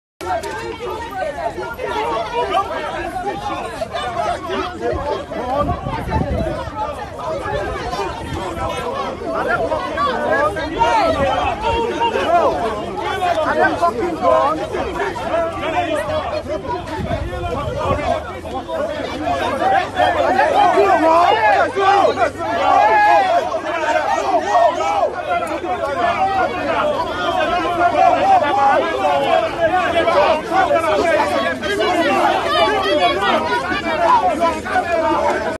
Protesters clash with security officers at COZA
Protesters clash with security officers as COZA protocol officers form barricade in front of the church